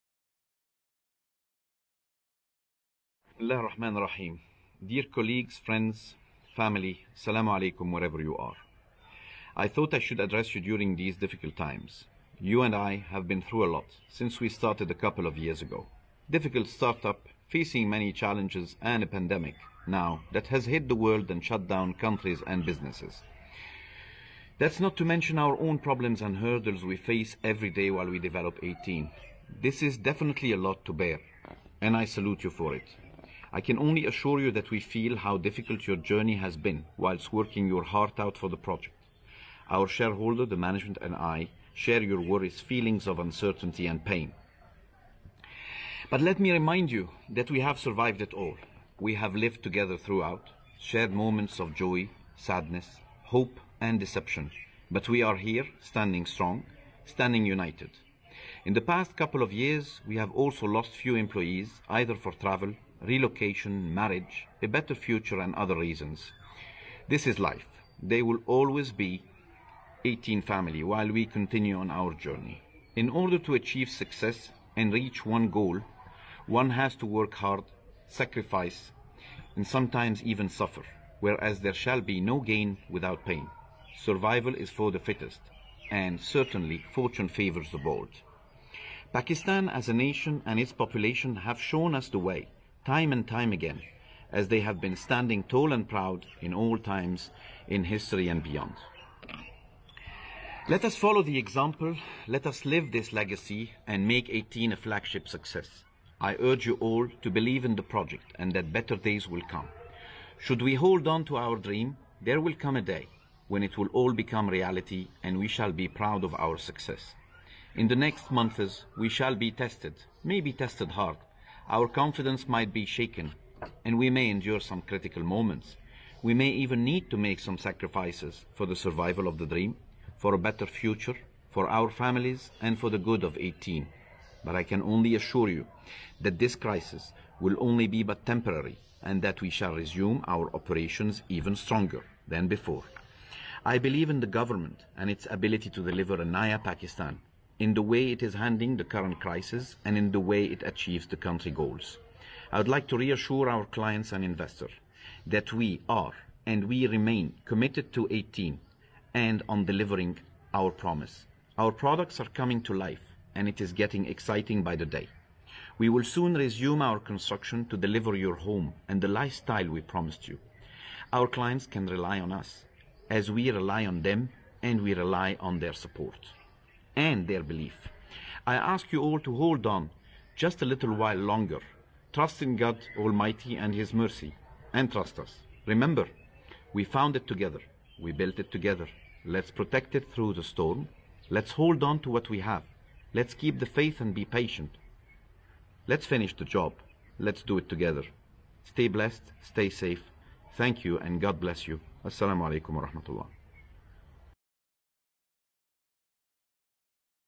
CEO's Message A promising and motivating message from Mr. Tarek Hamdy, CEO Eighteen,
CEO's Message
A promising and motivating message from Mr. Tarek Hamdy, CEO Eighteen, for the Eighteen Family - customers, stakeholders and employees.

We are Eighteen.